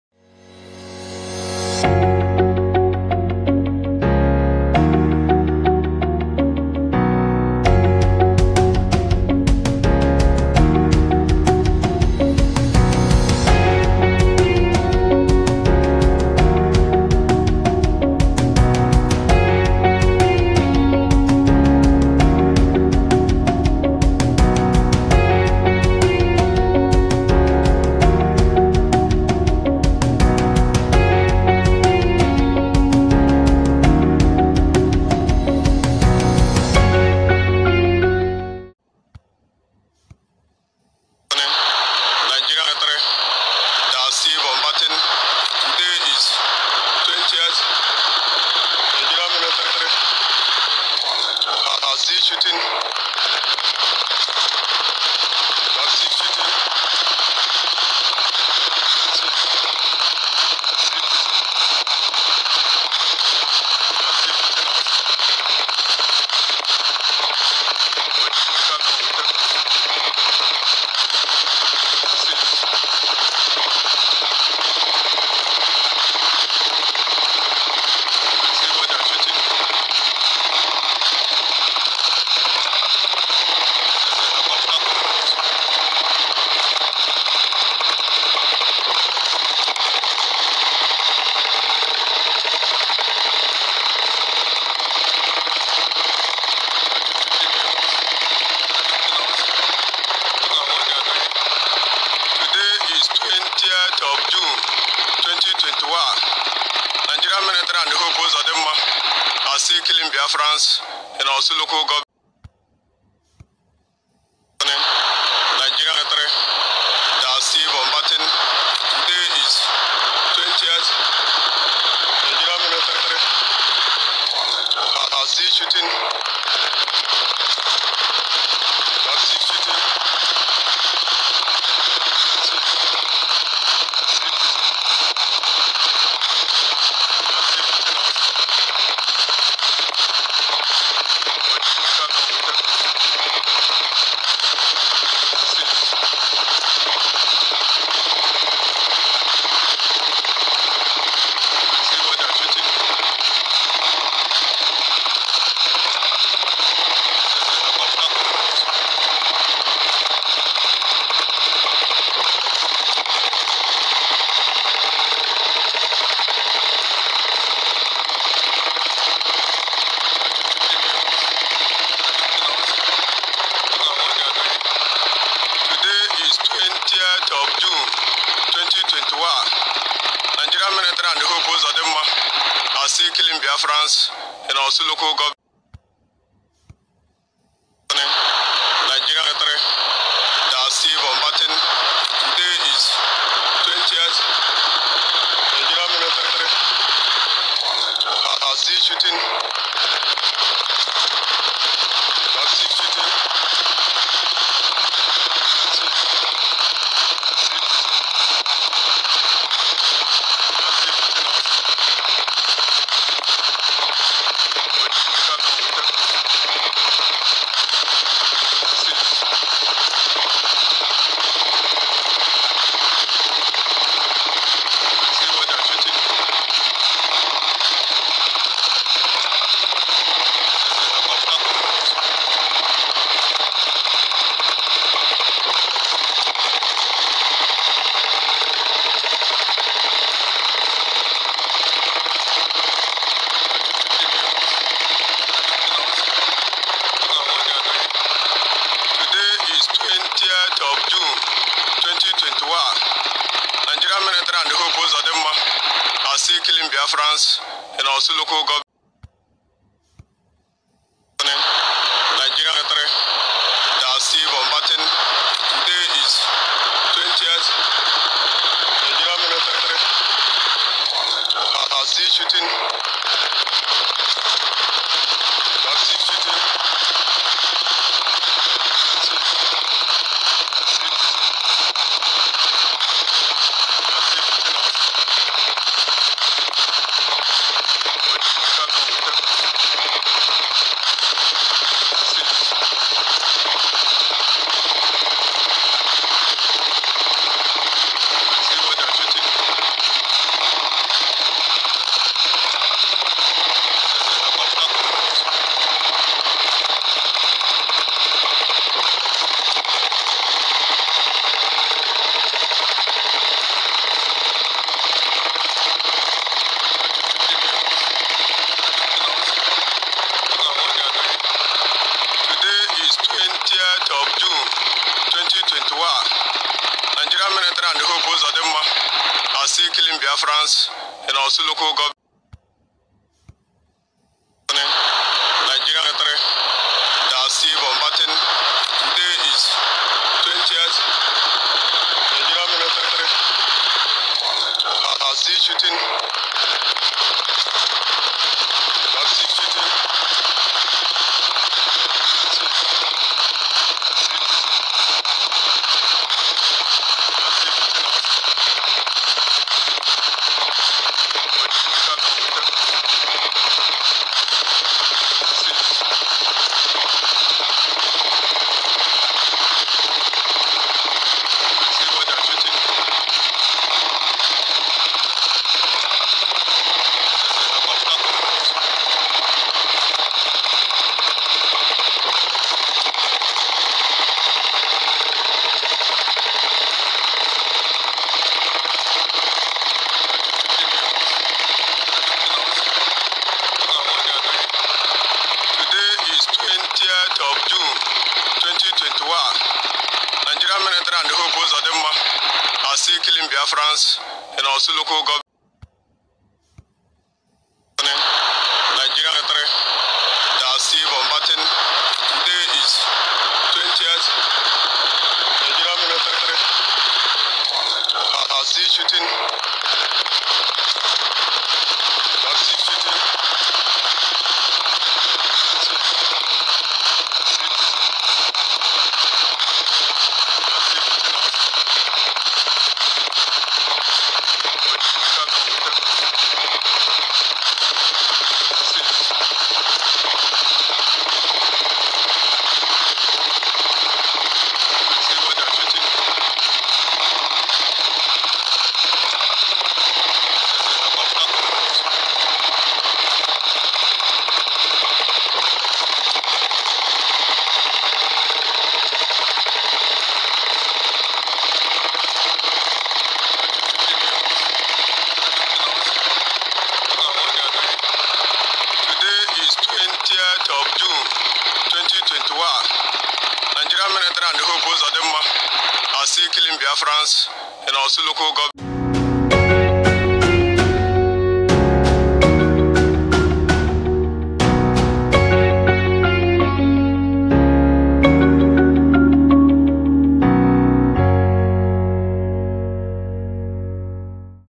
LIVE VIDEO: MILITÃRY AIRCRAFT SEARCHING 4 IPØB MEMBERS RETURNS TO IMO COMMUNITEIS CAUSES PÃÑIÇ,WATCH
NIG Ãrmy#Imo State